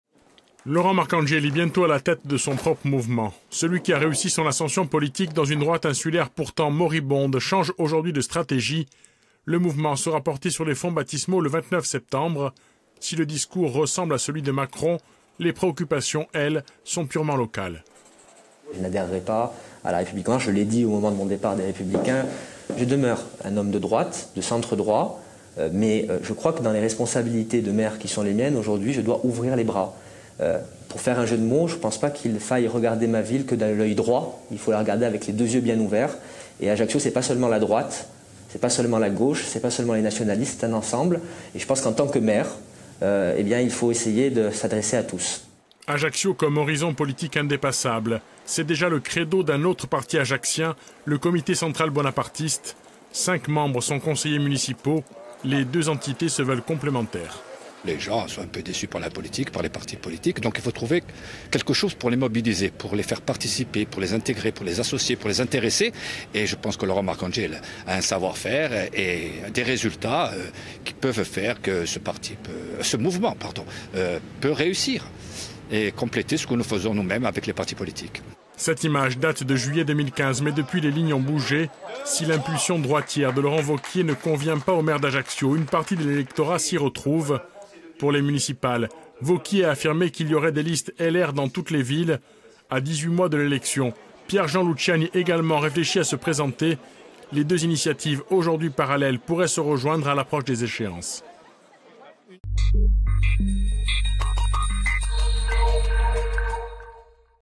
Ajaccio - Laurent Marcangeli bientôt à la tête de son propre mouvement
Le maire d'Ajaccio Laurent Marcangeli a annoncé la création d'une démarche politique propre à la ville, marquant ainsi un peu plus sa différence avec le mouvement Les Républicains.

Intervenants - Laurent Marcangeli, maire d'Ajaccio; André Villanova, président du Comité central bonapartiste;

Equipe - Dominique Moret, Franck Rombaldi.